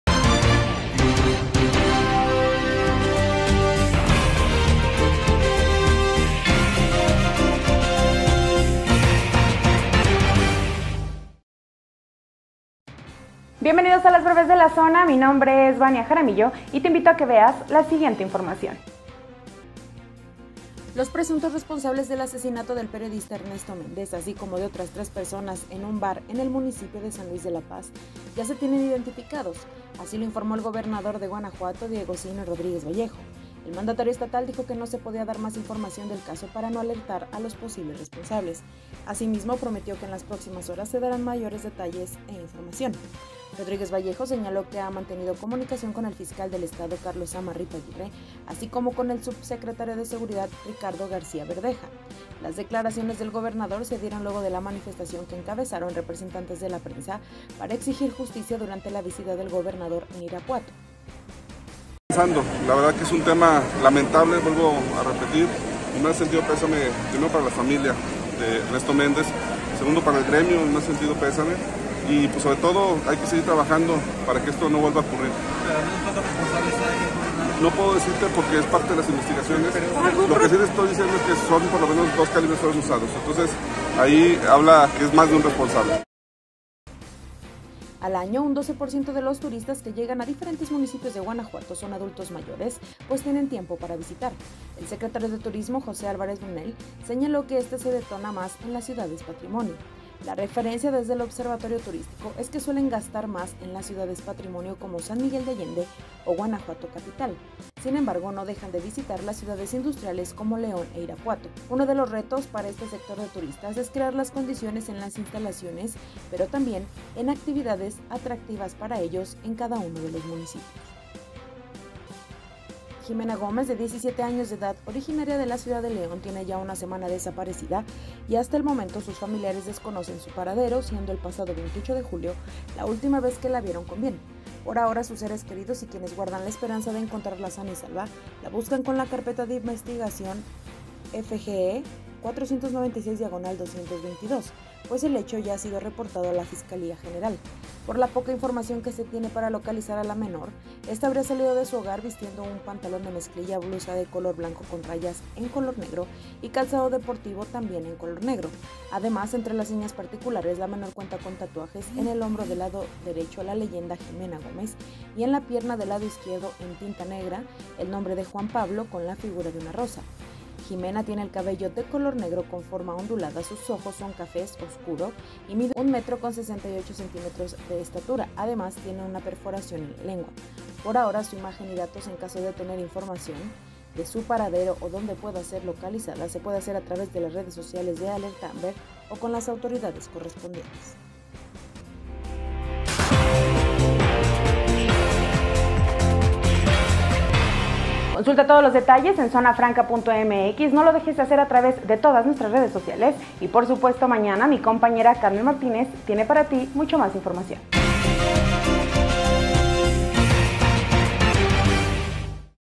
Ximena Gómez de 17 años de edad tiene ya una semana desaparecida | BrevesDeLaZona📰‼
Esta es la información destacada de este jueves:
-  Los presuntos responsables del asesinato del periodista Ernesto Méndez, así como de otras tres personas en un bar de San Luis de La Paz, ya se tienen identificados, así lo informó el gobernador del estado, Diego Sinhue Rodríguez Vallejo.
- Al año un 12% de los turistas que llegan a diferentes municipios de Guanajuato, son adultos mayores pues tienen tiempo para visitar.
-  Ximena Gómez de 17 años de edad, originaria de esta ciudad, tiene ya una semana desaparecida y hasta el momento sus familiares desconocen su paradero, siendo el pasado 28 de julio la última vez que la vieron con bien.